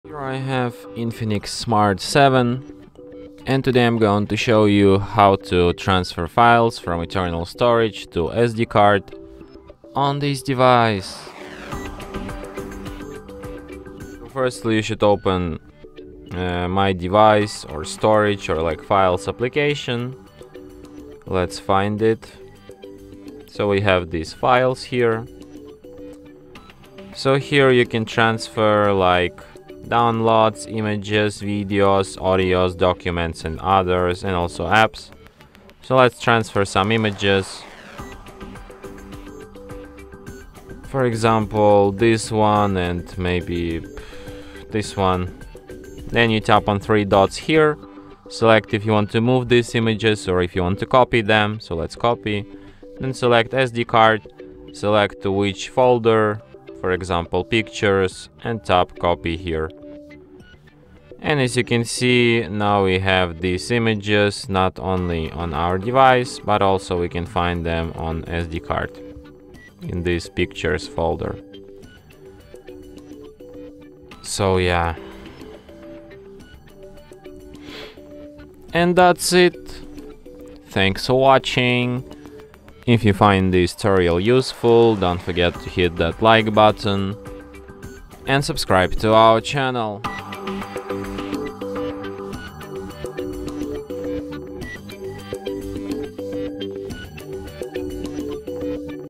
How to Transfer Files on INFINIX Smart 7
Welcome. Today we will show you how to transfer files from your SD card to your INFINIX Smart 7. Whether you want to move photos, images, videos, documents, or other files, stay here. Visit our YouTube channel if you want to know more about INFINIX Smart 7.

How to transfer files on INFINIX Smart 7? How to move files on INFINIX Smart 7?